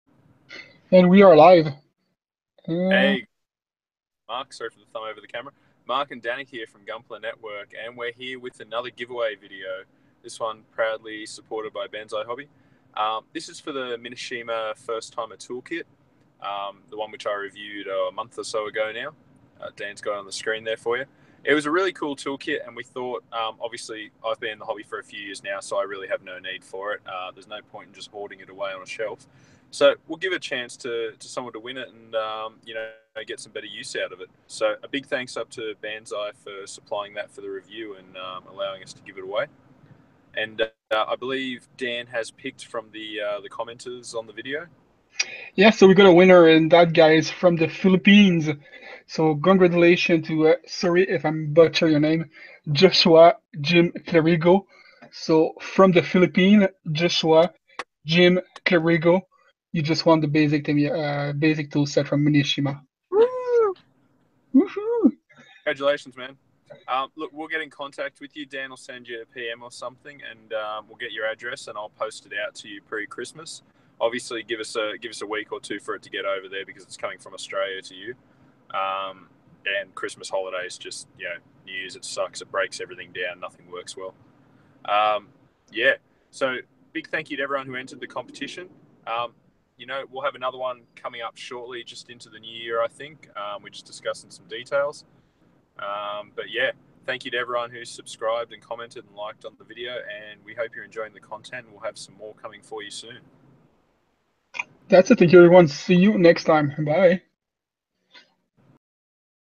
Giveaway Results: Mineshima First tool Set
It's beginning to look a lot like Christmas and that's why we are doing that small giveaway and give the opportunity to newcomer to the hobby to win their first tool set graciously provided by Banzai Hobby

Category